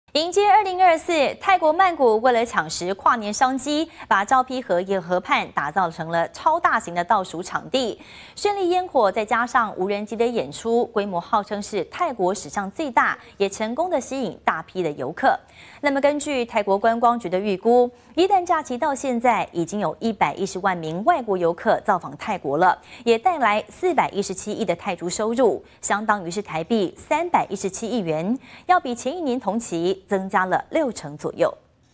5萬枚煙火綿延1.4公里施放 2千架無人機吸睛
迎接2024年，泰國曼谷為了搶食跨年商機，把昭披耶河畔打造成超大型倒數場地，炫麗的煙火、無機表演，規模號稱是泰國史上之最，成功吸引大量遊客。根據泰國觀光局預估，耶誕假期至今已經有110萬外國旅客造訪泰國，帶來417億泰銖的收入，約台幣317億，比前一年同期增加了6成。